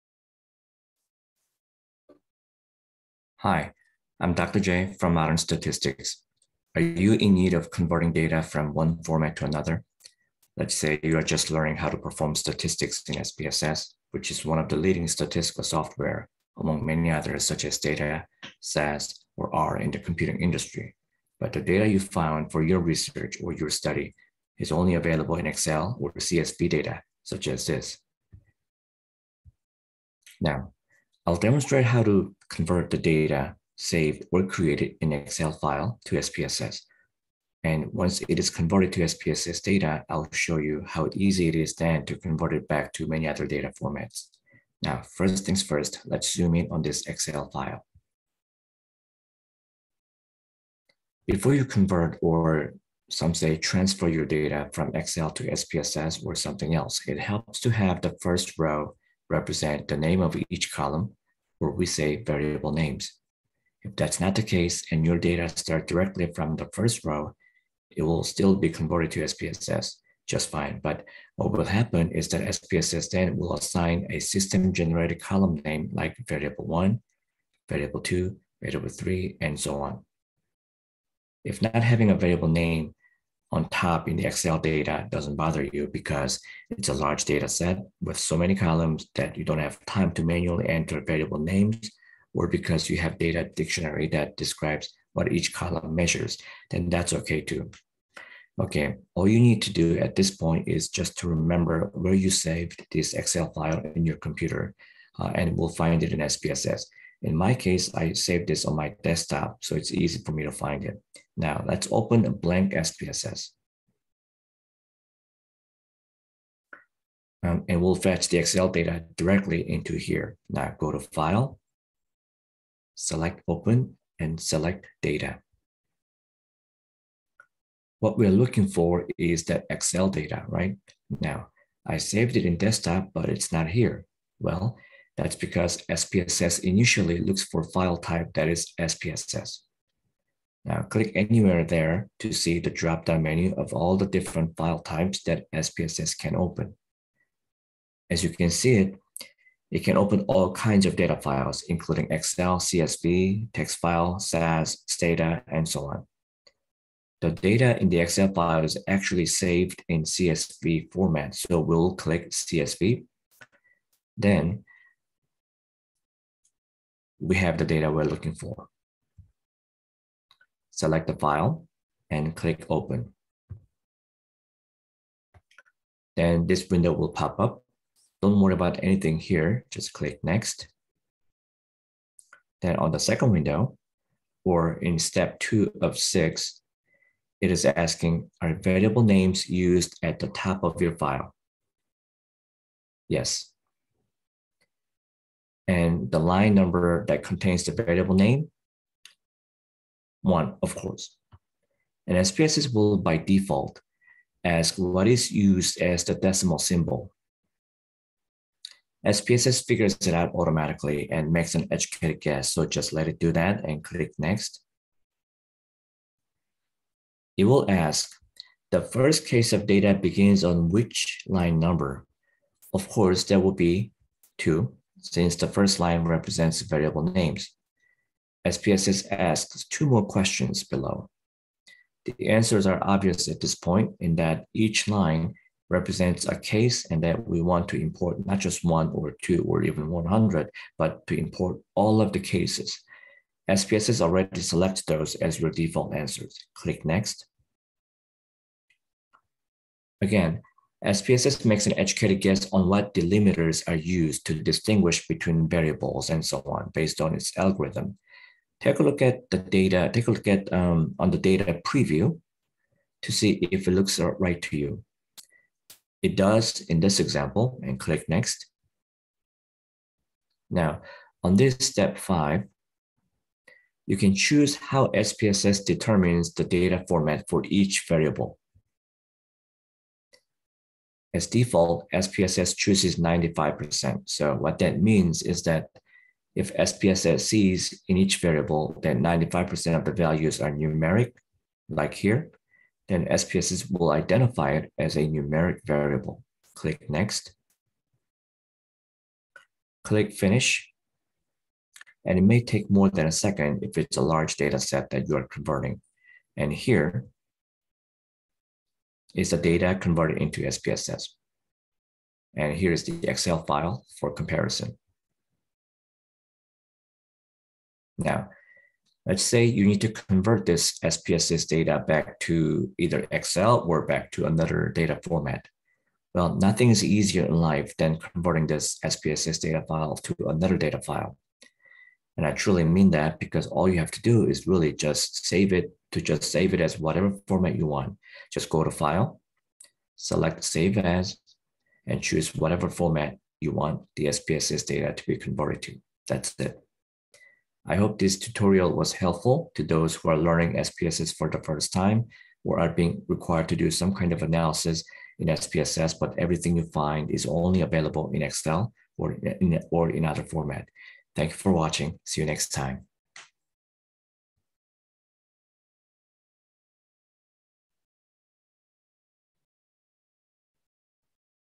SPSS Tutorial: Convert Any Data to SPSS
This short SPSS tutorial explains step by step how to convert Excel files to SPSS and from SPSS to many other data formats.